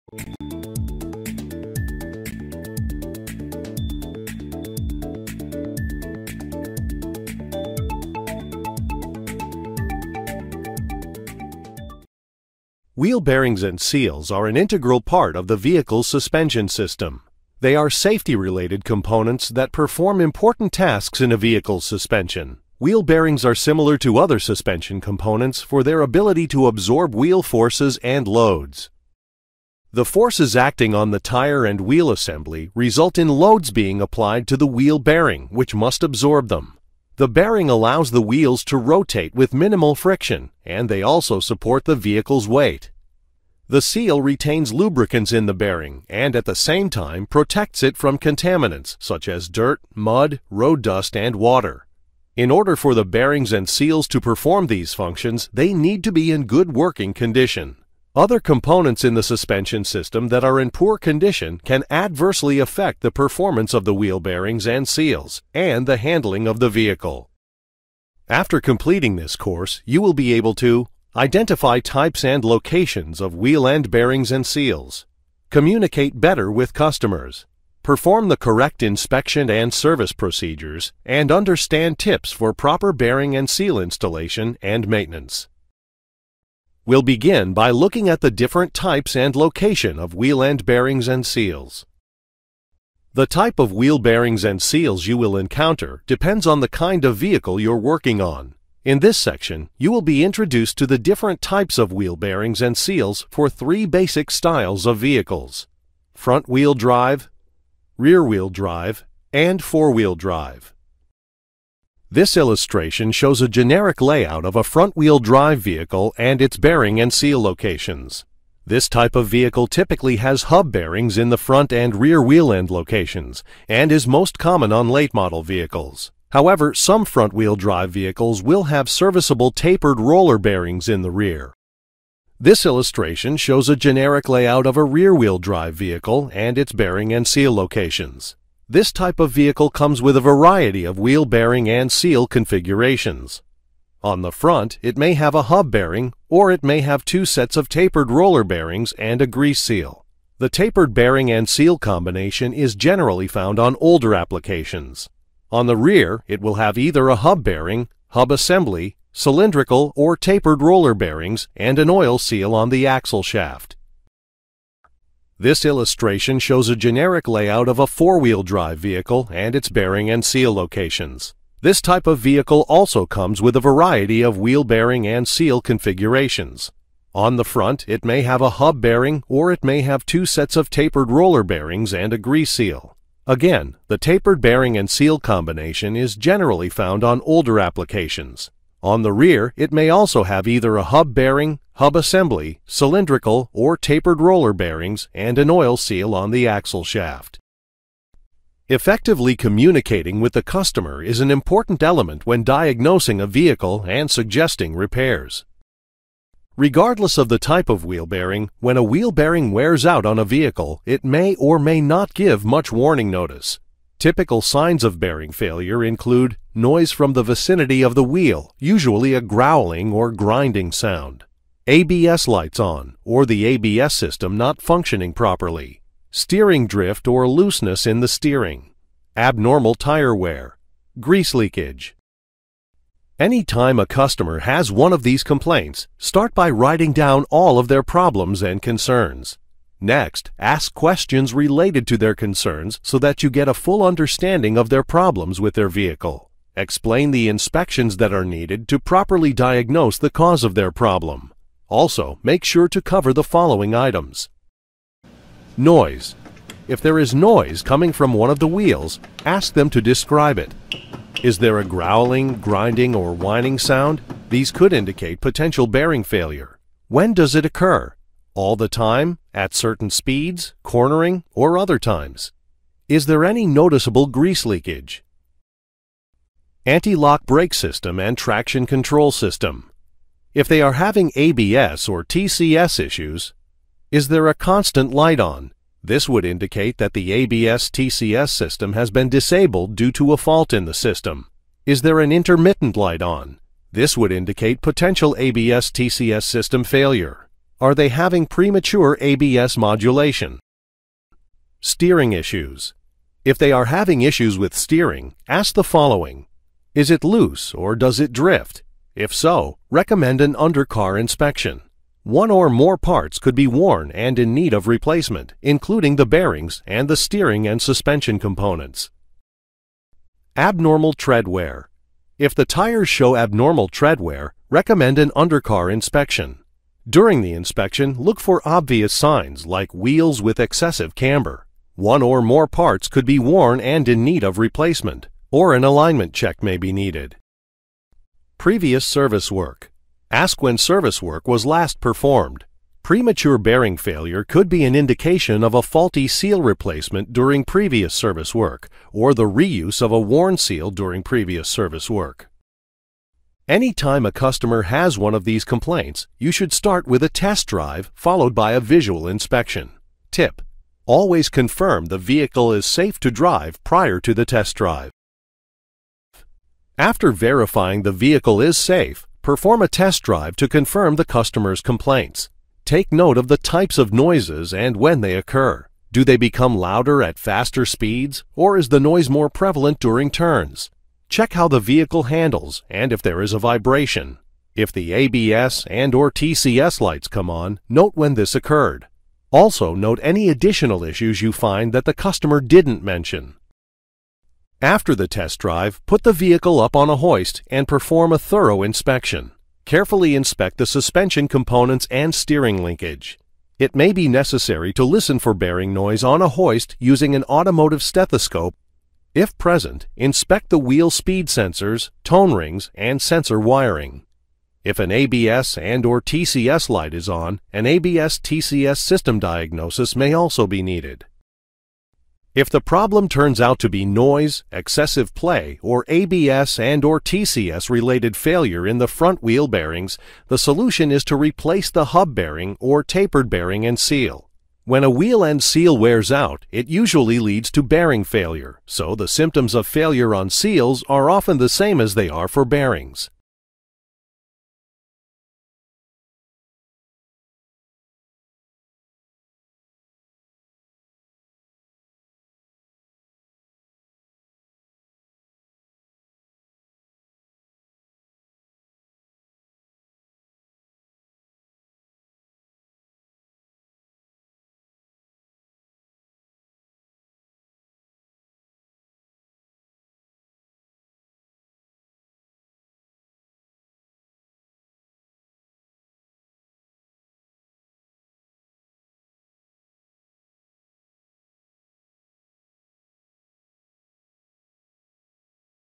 Chapter 1 - Introduction
Wheel bearings and seals are an integral part of the vehicles suspension system. They are safety related components that perform important tasks in a vehicle's suspension. Upon the completion of this course you will be able to identify the types and location of wheel end bearings and seals, perform the correct inspection and service procedures for these parts, understand tips for proper bearing and seal installation and maintenance, and explain the related service work that should be mentioned to the customer.